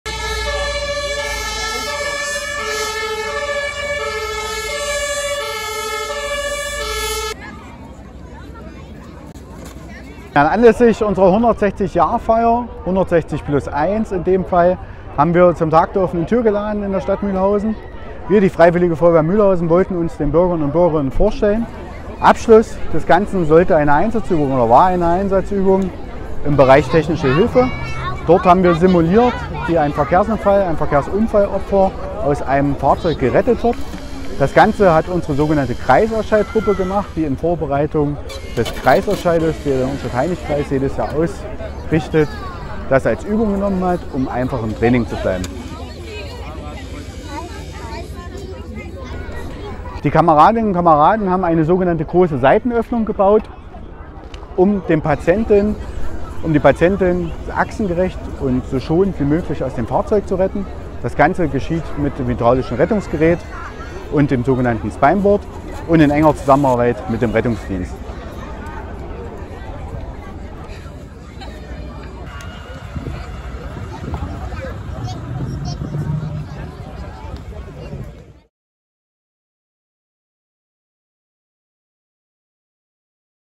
Tag der offenen Tür bei der Feuerwehr Mühlhausen
Am Tag der offenen Tür zum Jubiläum der Mühlhäuser Feuerwehr gab es für Groß und Klein viele spannende Einblicke in die Arbeit der Einsatzkräfte.